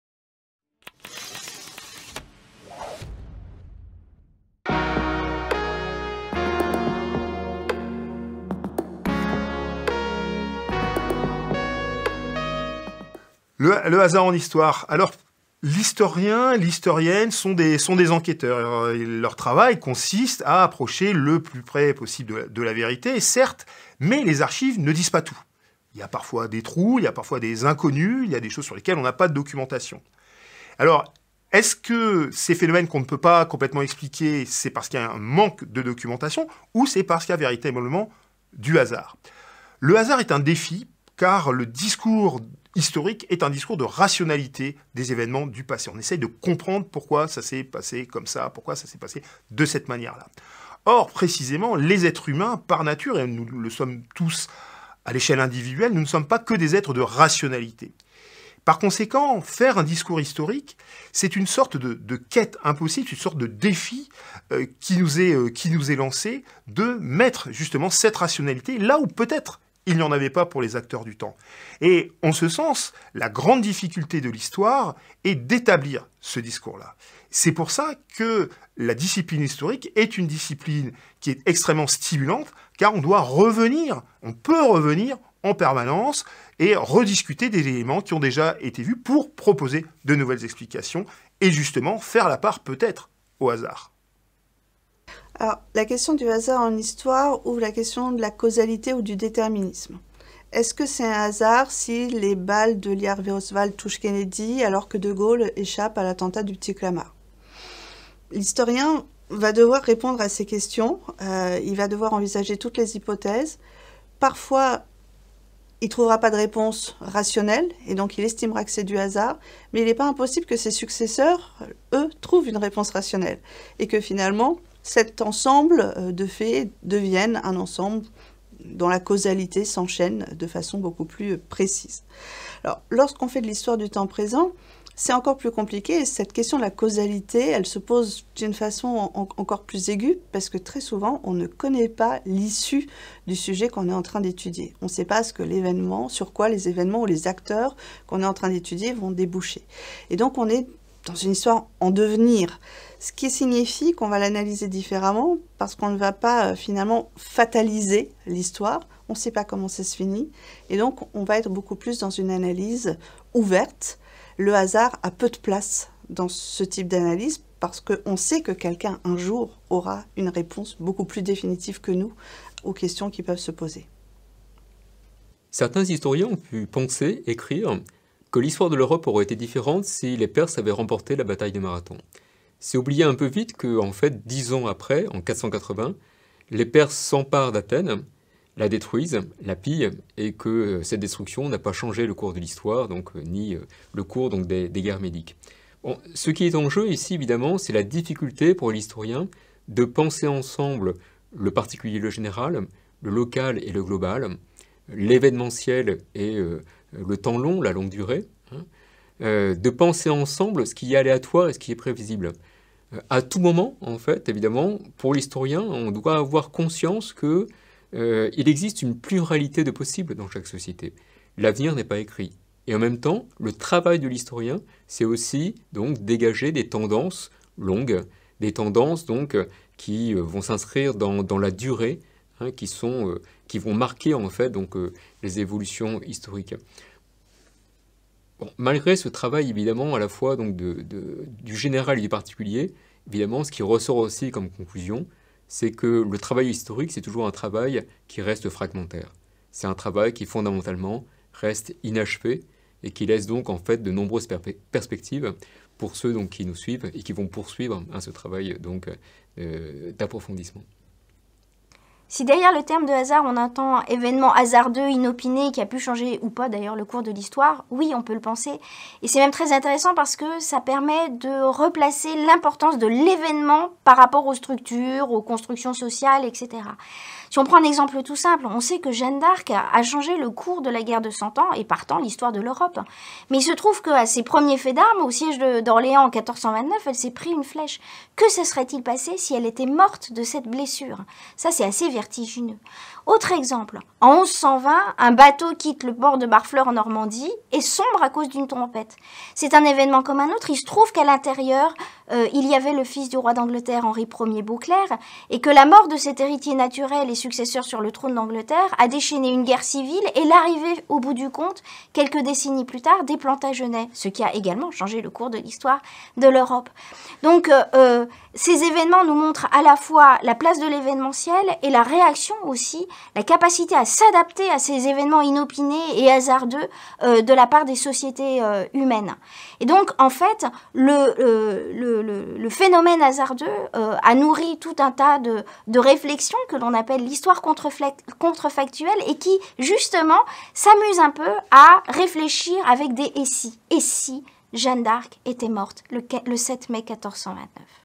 Faire de l'histoire - 3 : Hasard et histoire
Qu'est-ce qu'une démarche historique ? Quelle est la place du hasard dans l’histoire ? Peut-on faire de l’histoire de tout ? Comment l'histoire est-elle liée à la vérité ?

Afin d'initier les étudiants et étudiantes en histoire à la méthodologie et aux enjeux de la recherche historique, le Centre de Recherches en Histoire Internationale et Atlantique a invité quatre historiens et historiennes à se pencher sur ces questions. Leurs réflexions seront présentées sous forme d'une série de quatre vidéos, diffusées tous les quinze jours à partir de septembre 2024 sur la chaîne YouTube du CRHIA !

Merci à Annick Peters-Custot, Eric Schnakenbourg, Jenny Raflik-Grenouilleau et Jérôme Wilgaux pour leur intervention ainsi qu'au Pôle Audiovisuel et Multimedia de Nantes Université pour le tournage et le montage de cette vidéo.